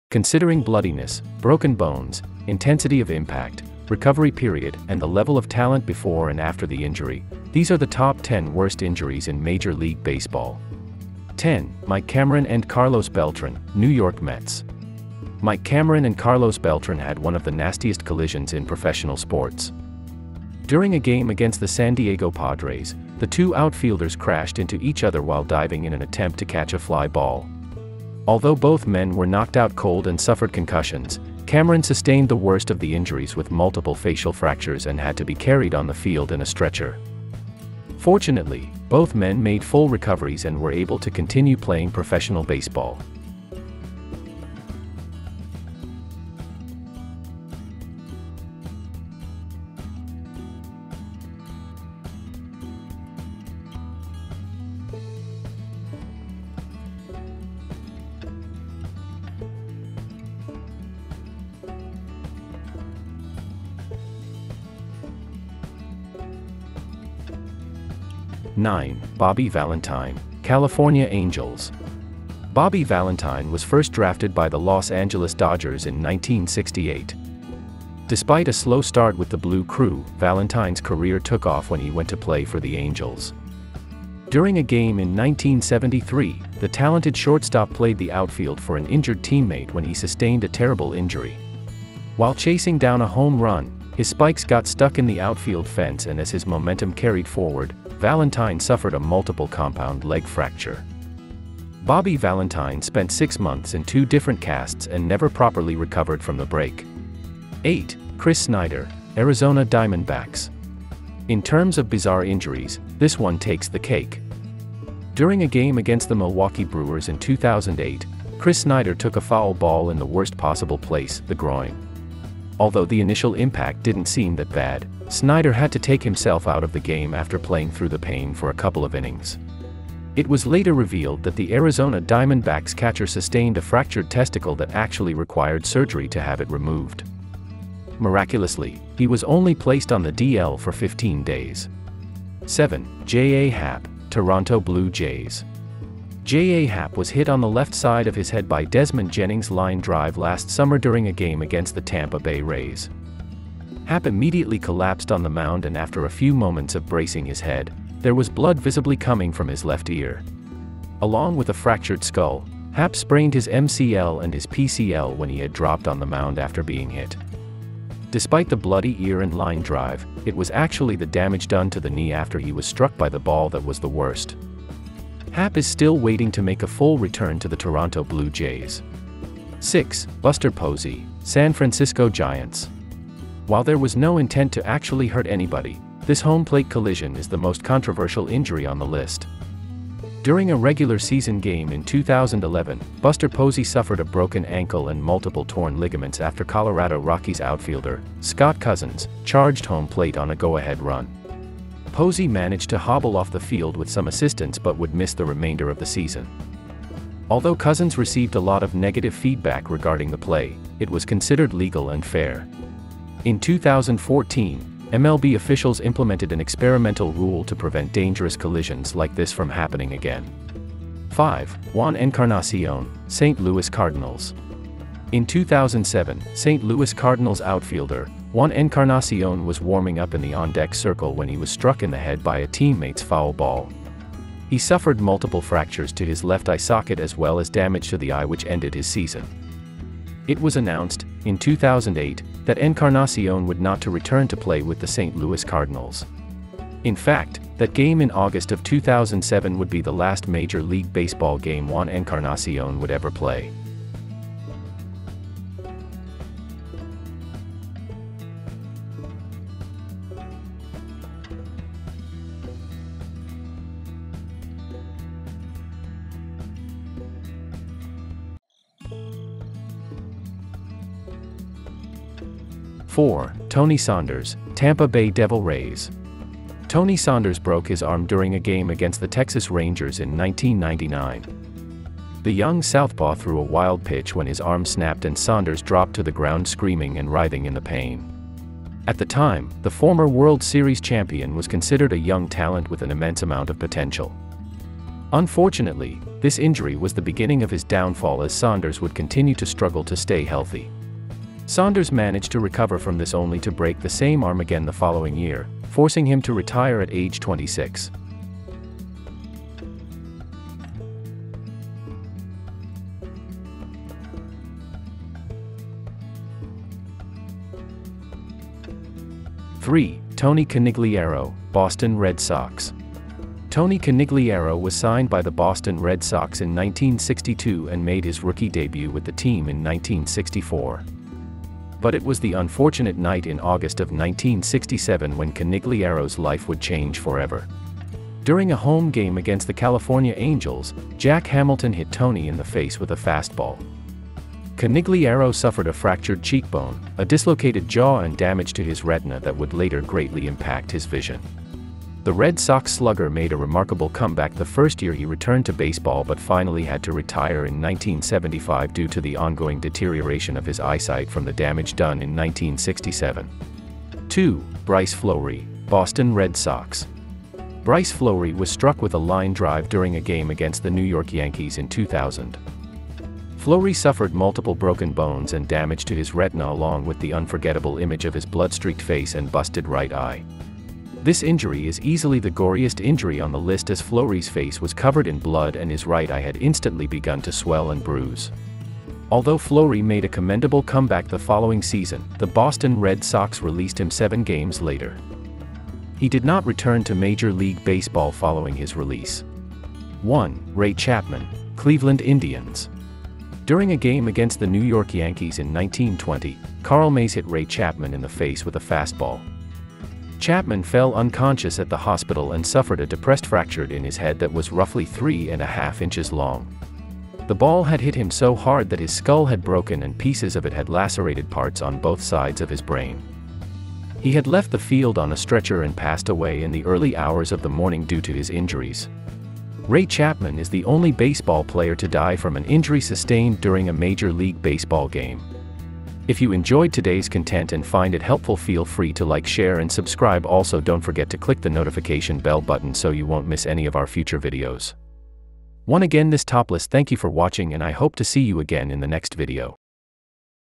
Top 10 Worst Injuries in Major League Baseball in history
Today we're going to take a look at the 10 Worst Injuries in Major League Baseball In History.

Our List:
00:00 Introduction
00:12 10. Mike Cameron _ Carlos Beltran
01:08 09. Bobby Valentine - California Angels
01:47 08. Chris Snyder
02:22 07. J.A. Happ - Toronto Blue Jays
02:59 06. Buster Posey - San Francisco Giants
03:42 05. Juan Encarnacion
04:33 04. Tony Saunders
05:20 03. Tony Conigliaro
06:04 02. Bryce Florie
06:40 01. Ray Chapman